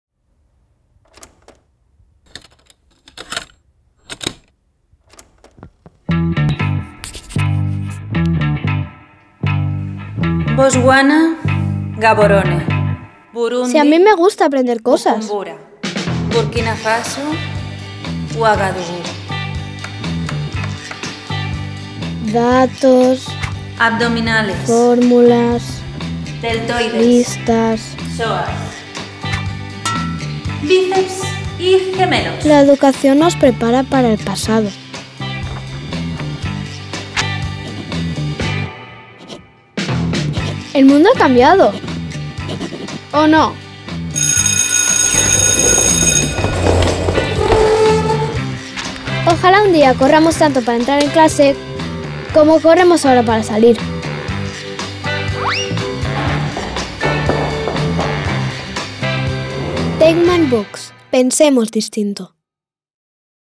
La educación ha cambiado, ¿o no?
Creemos que la educación debe adaptarse al siglo XXI y nuestra propuesta es hacerlo a través de las Inteligencias Múltiples. tekman Books es un proyecto educativo con los pies en la innovación y la cabeza en el cambio. Un viaje en el que los primeros pasos marcan la diferencia.